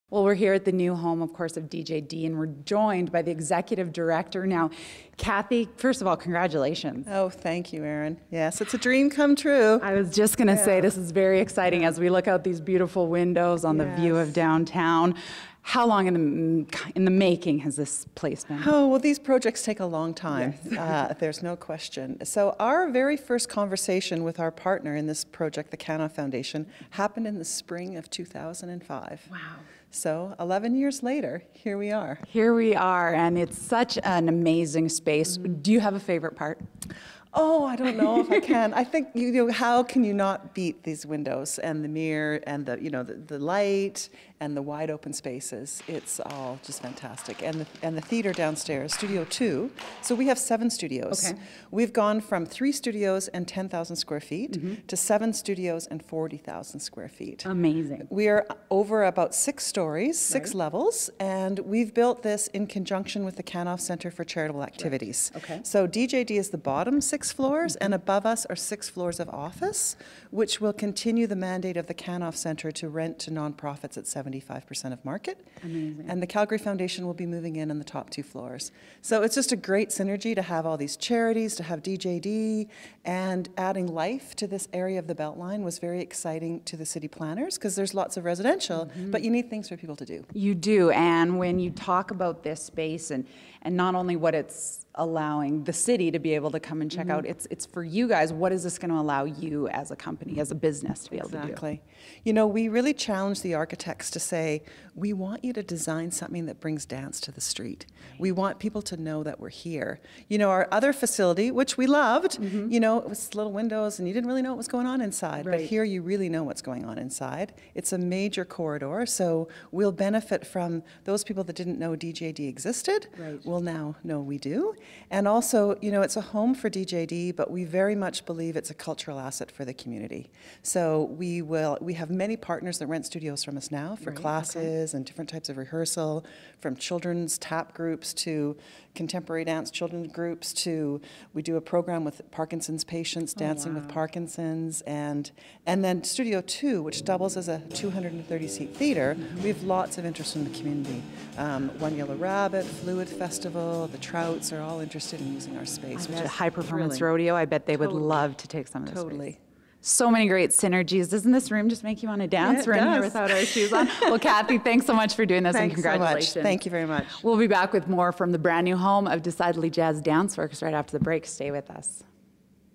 Kathi Sundstrom / Executive Director, DJD Dance Centre
Kathi Sundstrom / Executive Director, DJD Dance Centre, shares the excitement of the new dance centre with Erin Strate.